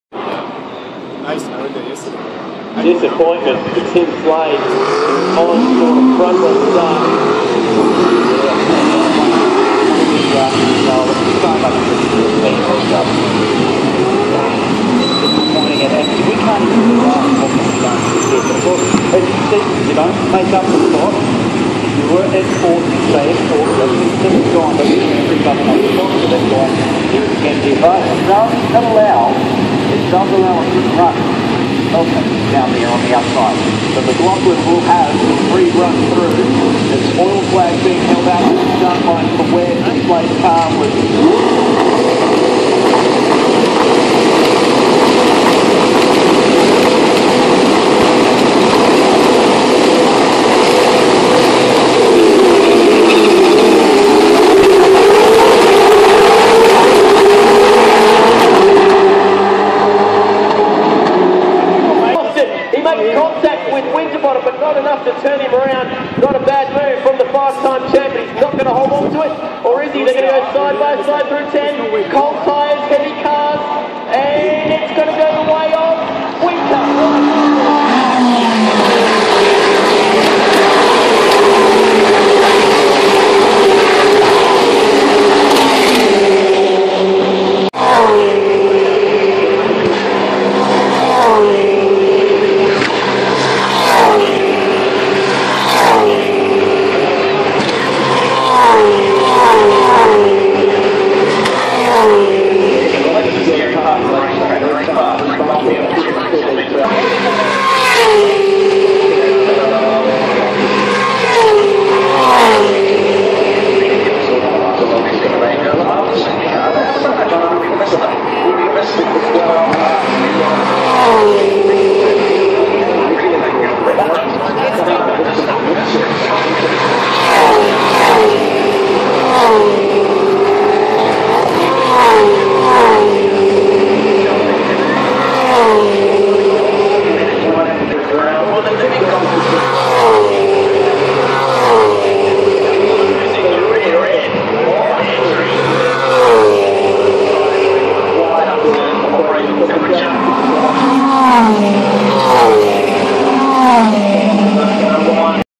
My views from the ITM 400 V8 Supercars race
My views from the V8 Suoercar race at pukekohe on sunday 27th april 2014